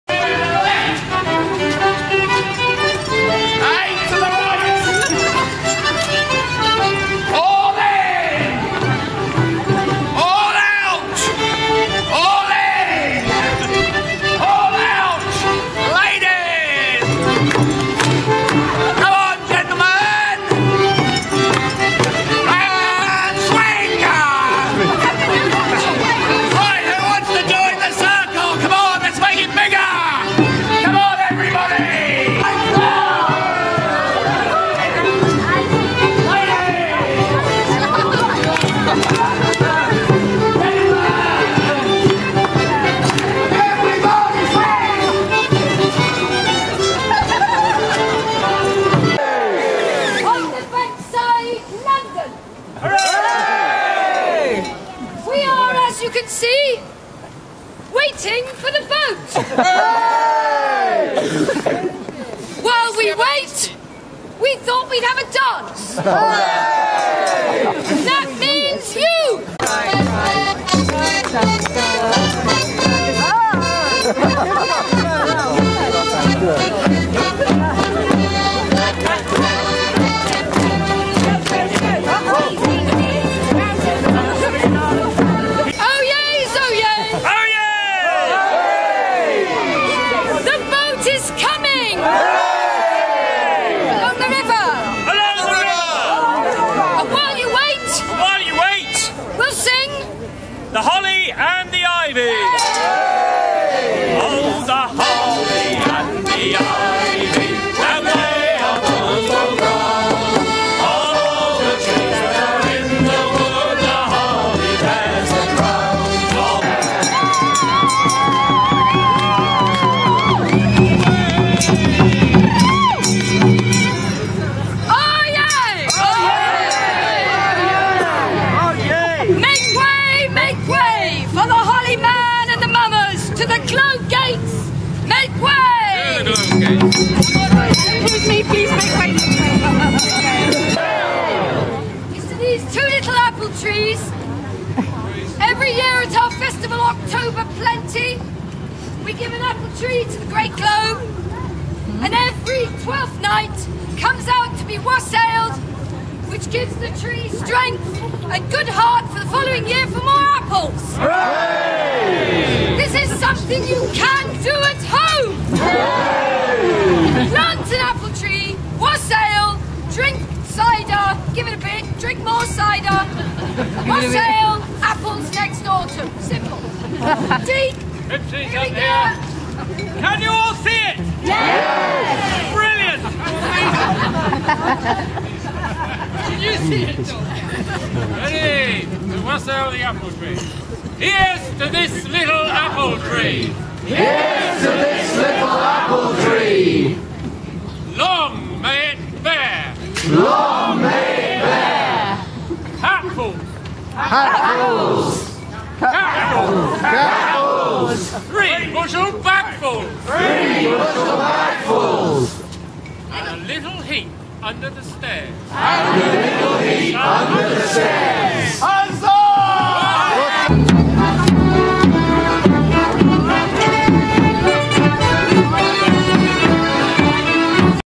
Twelfth Night - Wassail to the Globe theatre in London from the Lions part (3rd January 2010) 1 of 3
The Lions Part celebrate Twelfth Night at the Shakespeare Globe theatre in London.

Celebrations started with a Wassail to the boat (that brought the Green Man), to the Globe theatre itself, to an apple tree that grows at the Globe and to the George Inn, a public house just a short walk from the Globe.

The Lions part also performed a Mummer's play as part of the celebrations, along with the crowning of  a King Bean and Queen Pea using Twelfth Night Cakes, followed by a procession to the George Inn.

The cast of the Mummers' Play: Father Christmas: Peter Kenny / St George: Michael Palmer / Prudence:Dorothy Lawrence / Turkish Knight:Scott Ainslie / Doctor:Duncan Law / Gill Finney:Sarah Finch / Cleverlegs:Tim Laycok / Beelzebub:Stephen Ley / Twelfth Bake: Sonia Ritter / Green Man:David Risley / White Bear:Tamsin Lewis